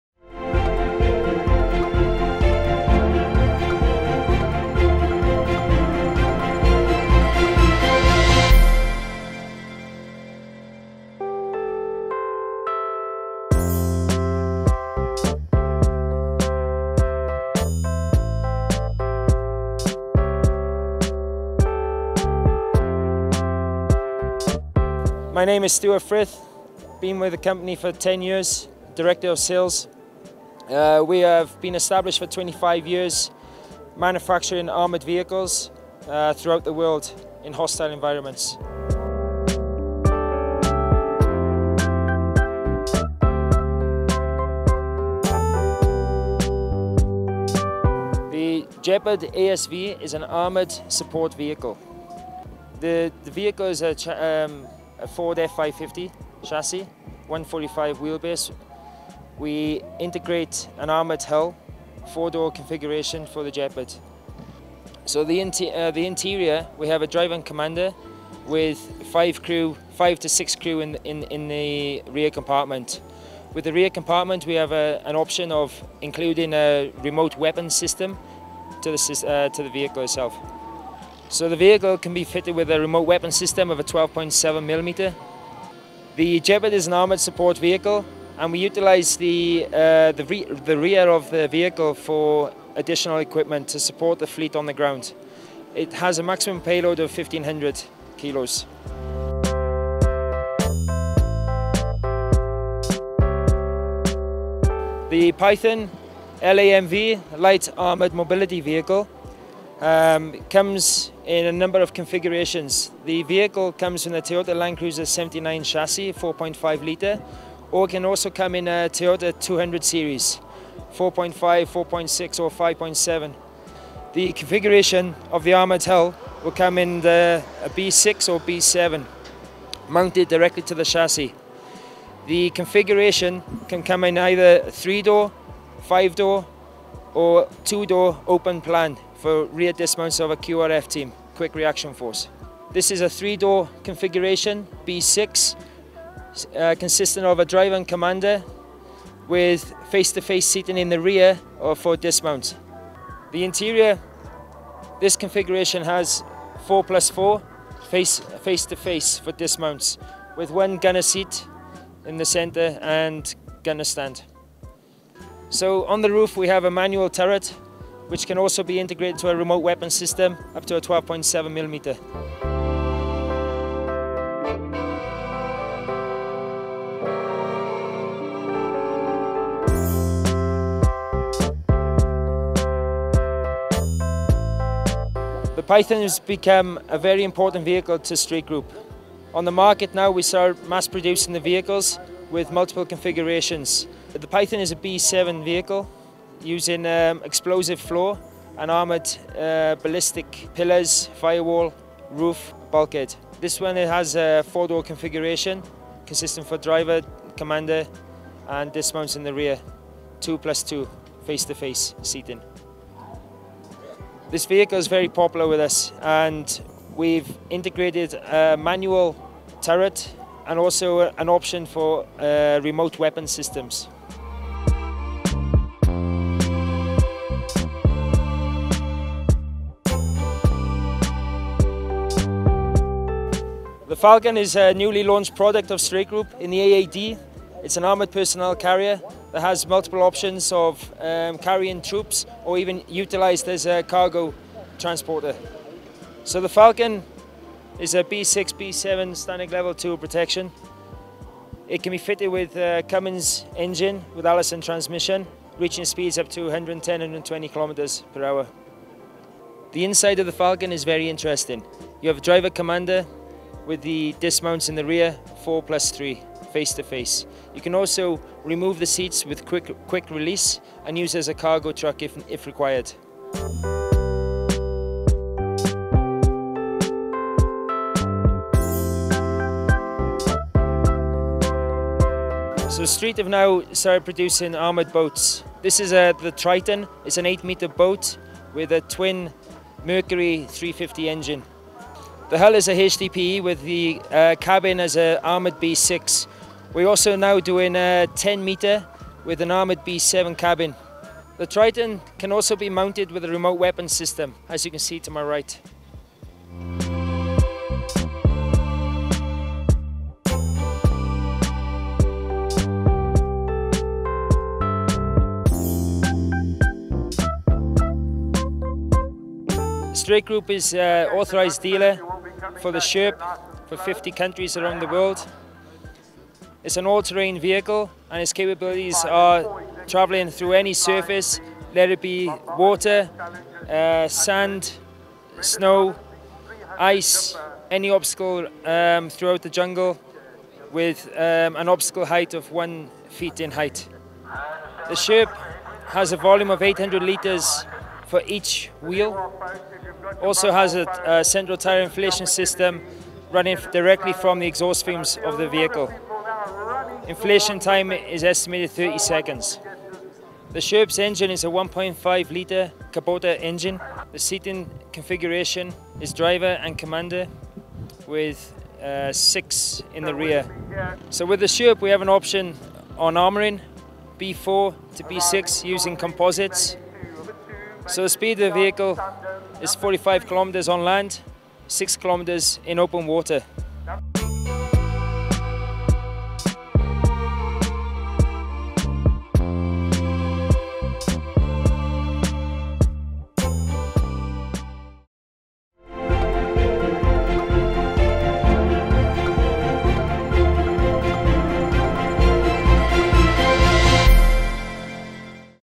AAD 2018 Streit Group presents its full range of armored vehicles APC static & live demonstration
At AAD 2018, the UAE-based Company Streit Group, a global leader in the manufacturing of combat vehicles presents its full range of armoured vehicles personnel carrier APC including the Gepard ASV, Falcon APC, Python, Python LAMV and the SHERP all-terrain amphibious vehicle in live demonstration.

Read full news about Streit Group at ADD 2018 on this